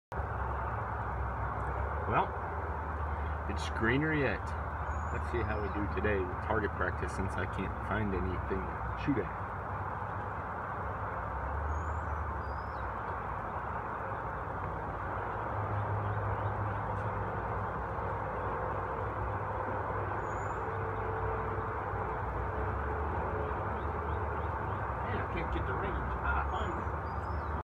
Greener yet
Just target practice - I am out of memory already? I wonder what kind of data base this phone is building cuz I keep running out of memory quicker and quicker. Looks like I gotta get my 12 year old to fix it!